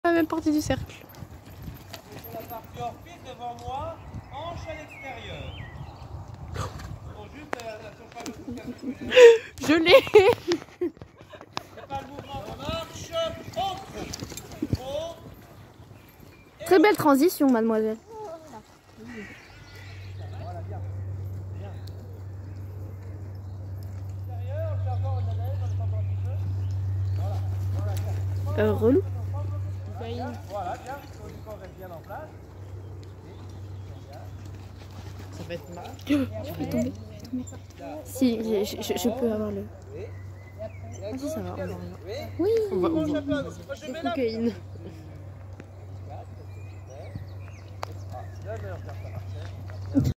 Cocaïne, mes 20/06/22
Cocaïne d'elvange
trot sur cercle
galop rênes longues sur cercle
allongement sur le demi cercle
galop/trot entre les 1eres barres/galop entre les 2emes

merci à Marie et candice pour les vidéos!